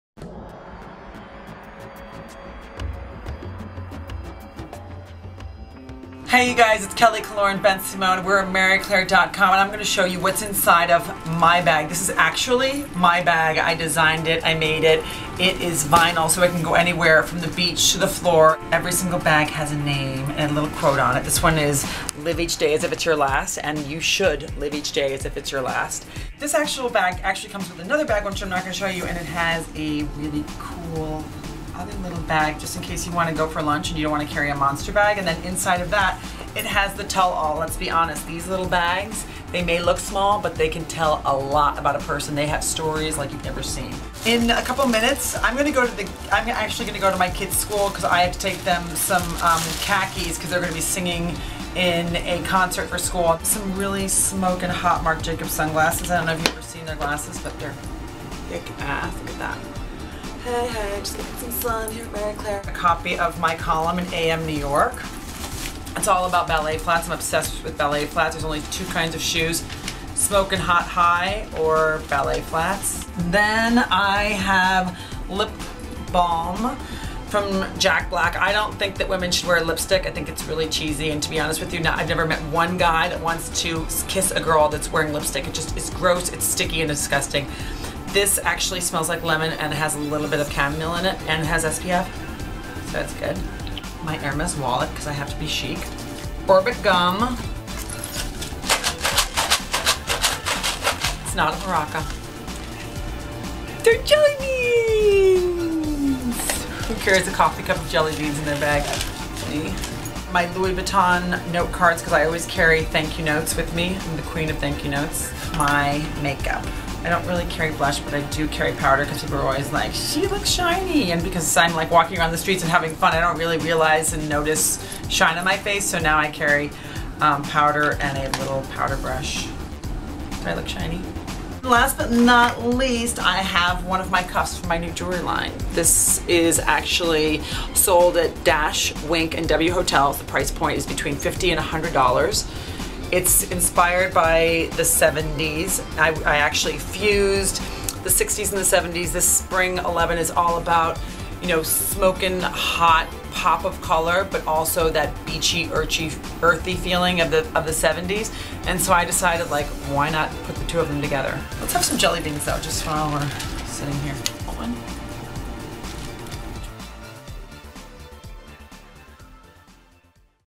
What's in Kelly Killoren Bensimon's Bag?
The Real Housewives of New York City star spills the contents of her bag — which she designed herself! — while giving out beauty and footwear advice, snacking on jelly beans, and talking about her new jewelry collection.